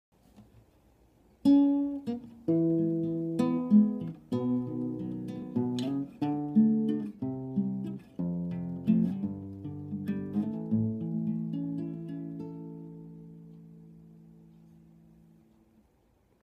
Christmas coffee mug challenge
Improvised tune that begins with the first three notes on Patrick's coffee mug. Noice.